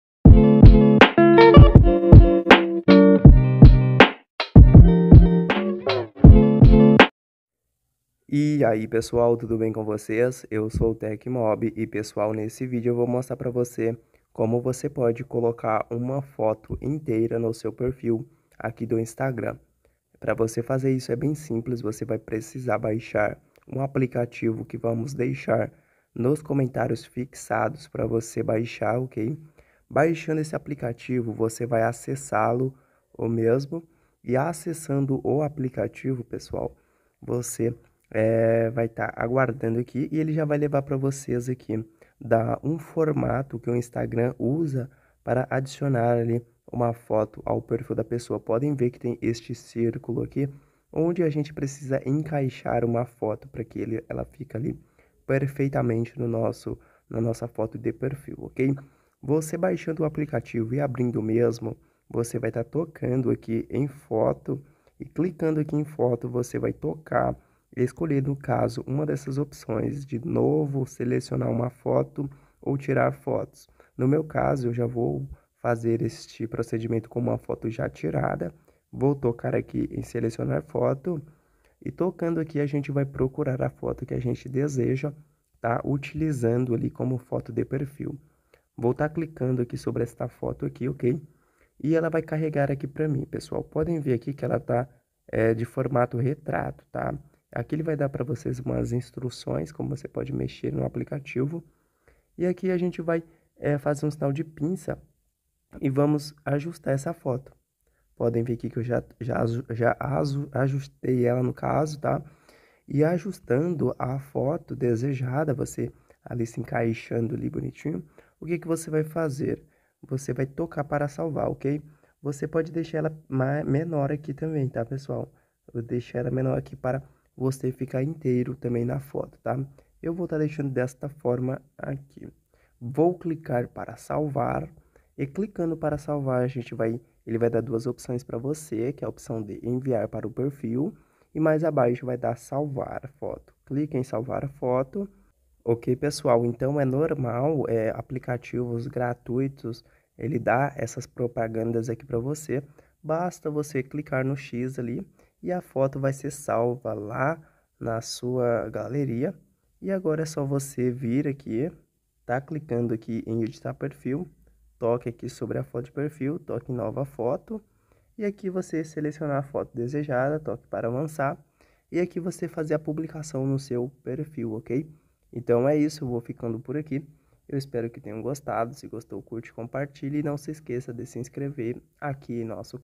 Como colocar FOTO INTEIRA no PERFIL do Instagram
➥ Título: Como colocar foto inteira no perfil do Instagram? Nesse vídeo você irá aprender como você pode adicionar foto inteira no seu perfil do Instagram! Assista a este vídeo para não ter dúvidas em como botar foto inteira no perfil do Instagram.

x
x
x
✅ Contato Profissional:
x
✓ Foto inteira no perfil do Instagram